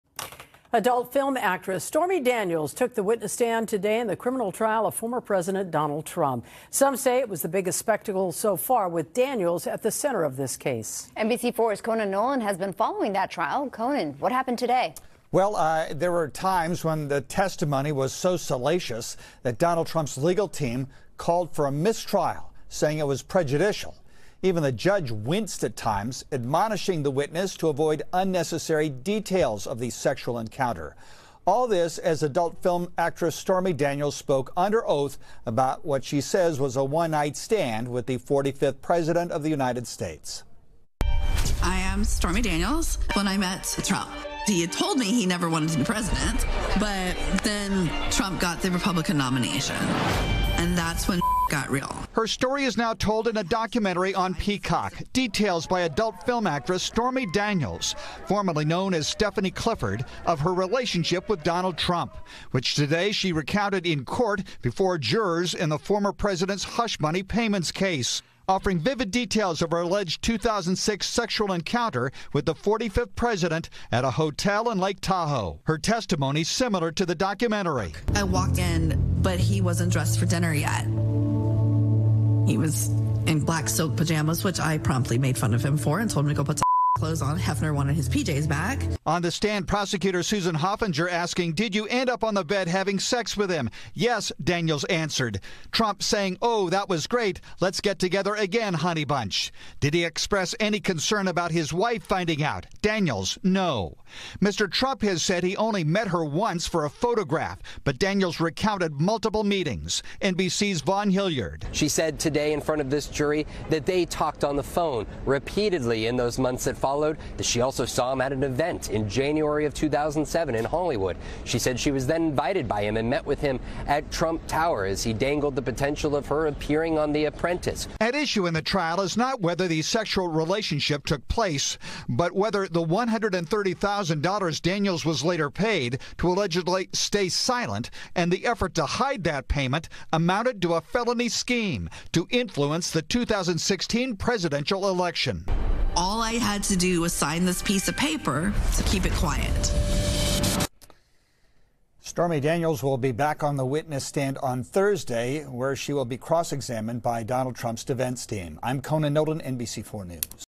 Here's what Stormy Daniels said during Trump's hush money trial
Stormy Daniel's testimony was so salacious that Donald Trump's legal team called for a mistrial on Tuesday. NBC Los Angeles' Conan Nolan reports.